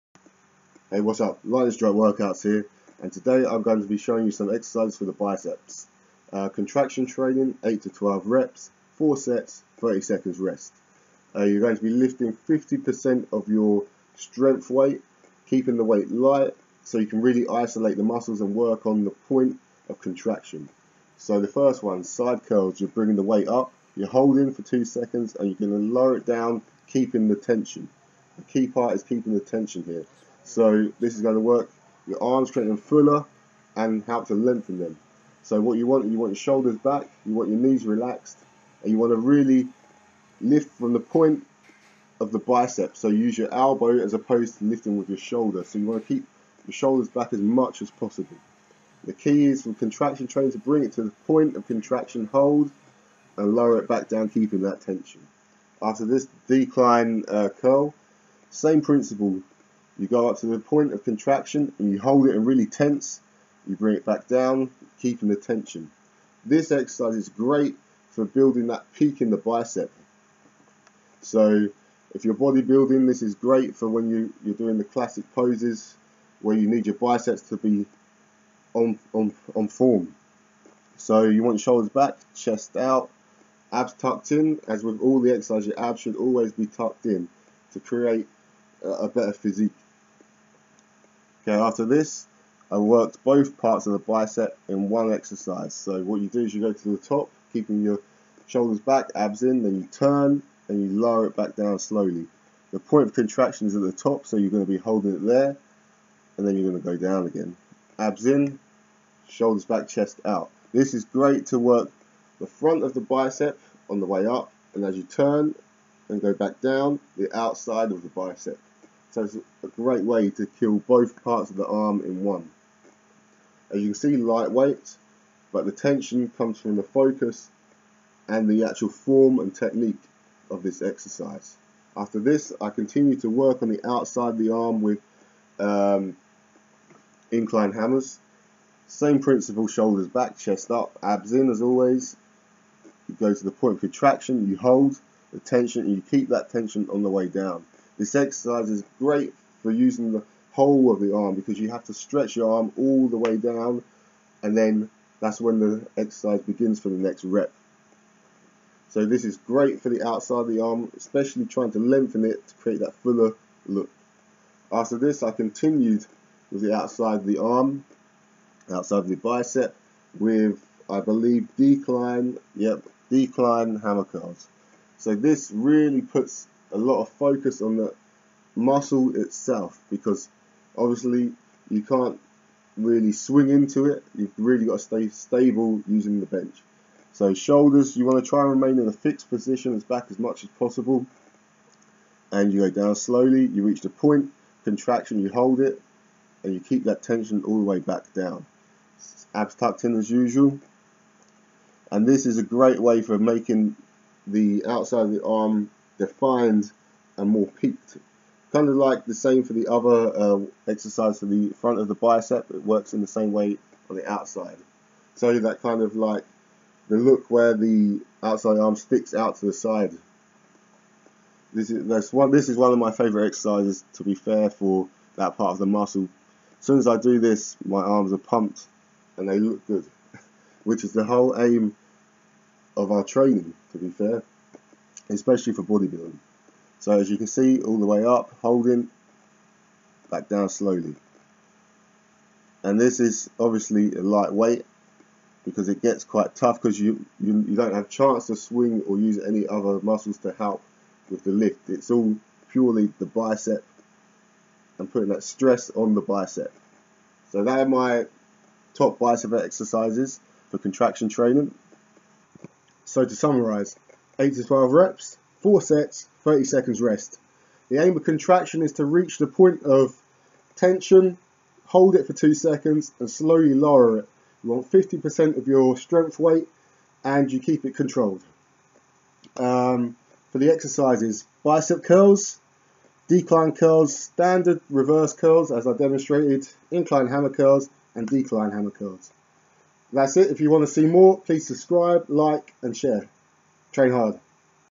TOP BODYBUILDING EXERCISES TO BUILD BIGGER BICEPS/ CONTRACTION TRAINING
My favourite exercises to build bigger, more defined biceps during contraction week!

Any questions? feel free to put them in the comment section and I'll try my best to help out!